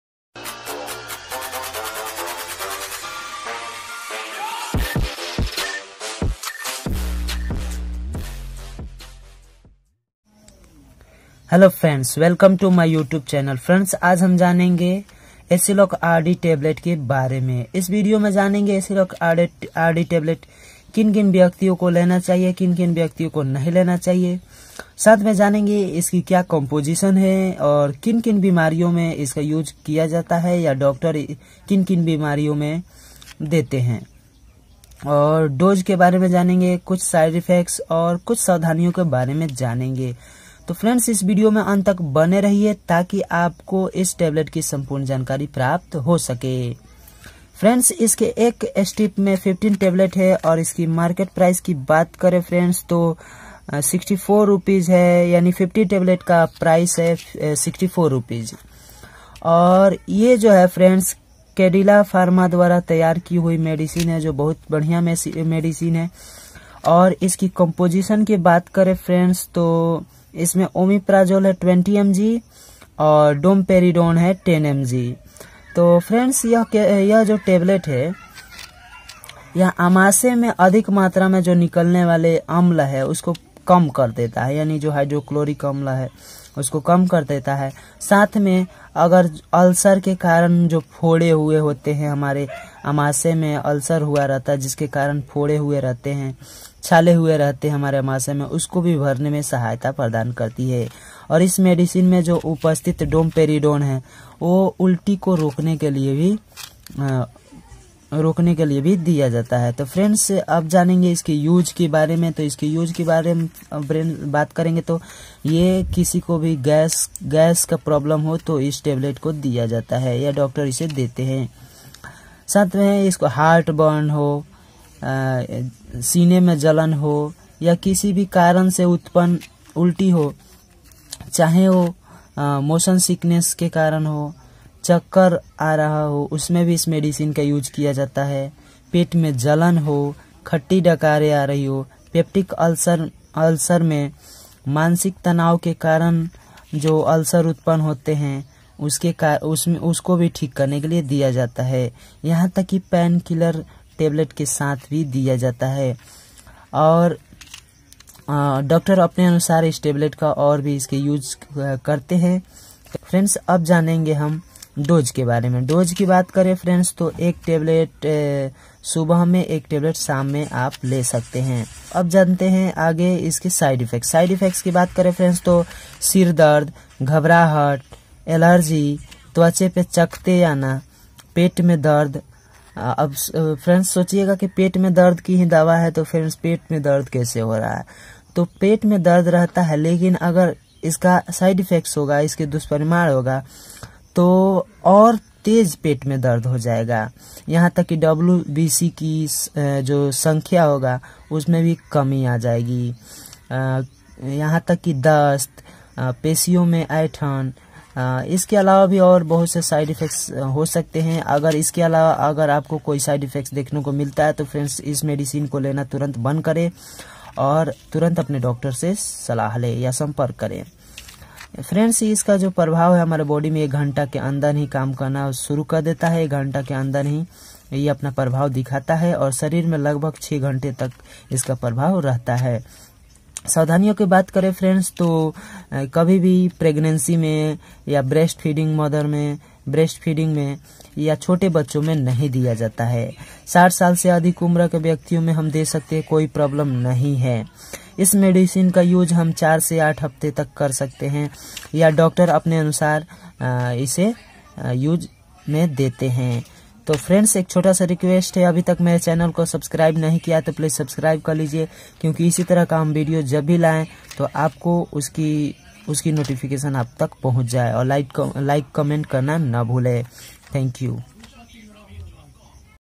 Aciloc rd tablet uses in hindi | omeprazole and domperidone tablet uses in hindi
गाँवो में सबसे ज्यादा इस्तेमाल की जाने वाली गैस, पेट दर्द की दवा /Aciloc RD/Omeprazole+Domperidone

About this video -Friends hum is video me Aciloc RD tablet ki sampurn jankari janege.